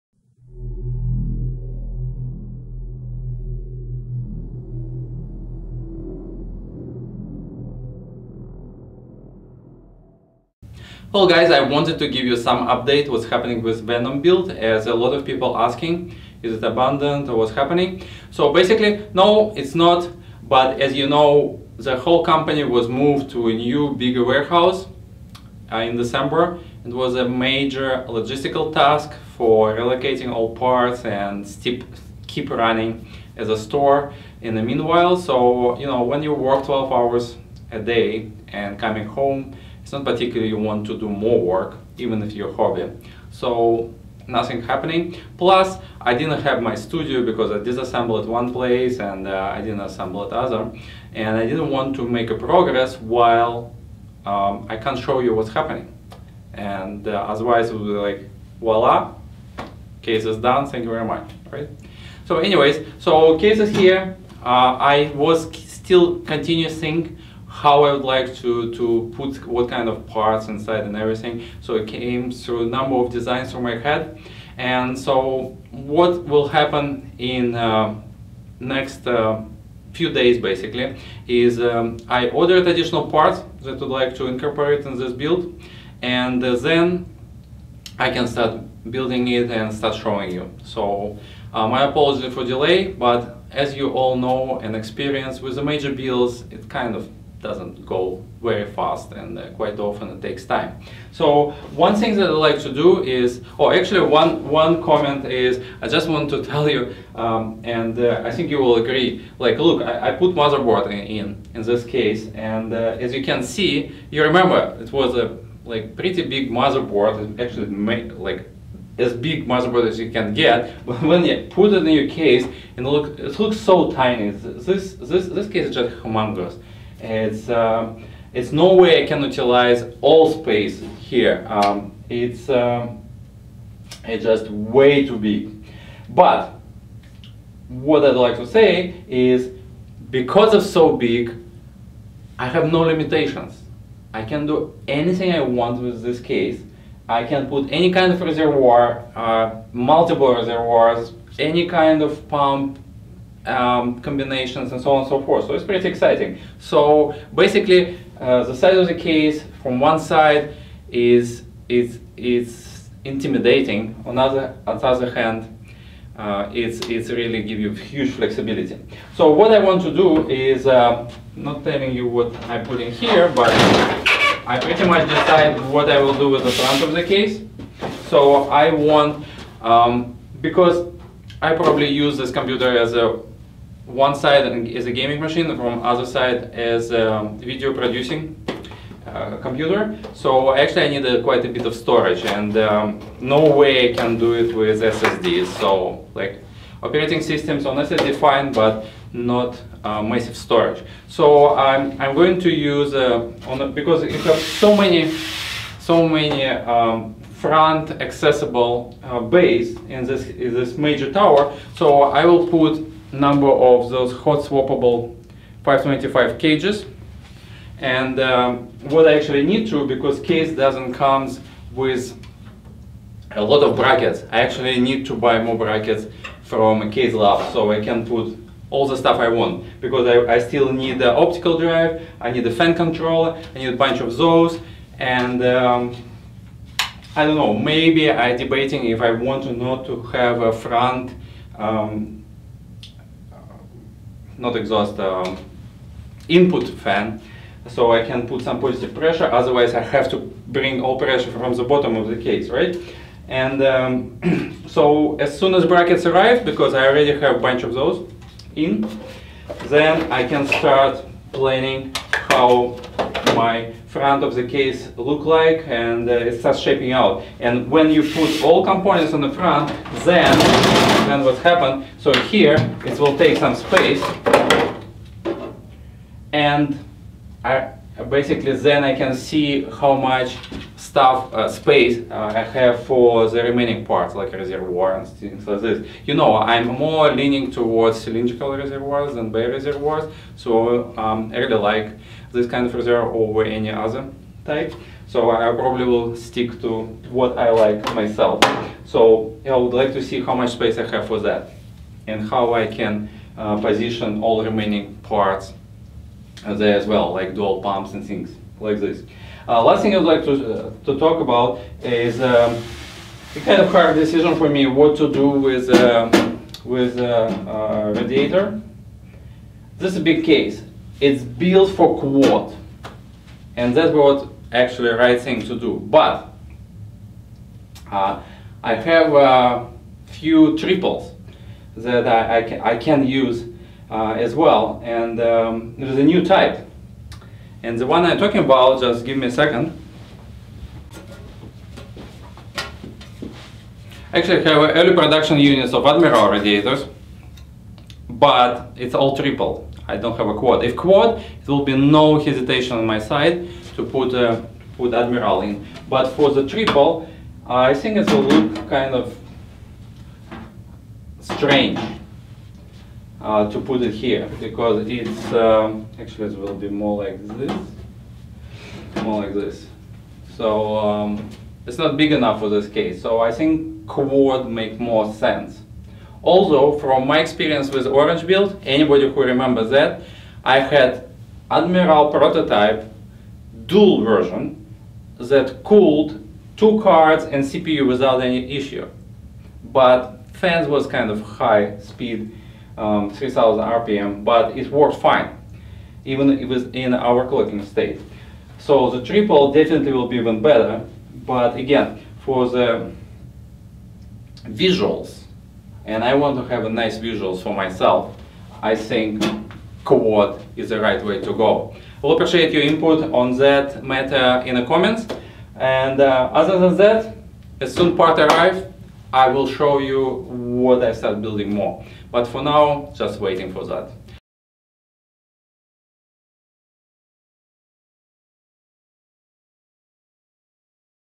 Venom SMH10 Build Update - Case's front planning + debating on using Admiral Radiator
What is lying ahead for Venom Build. Planning for bay devices and radiator.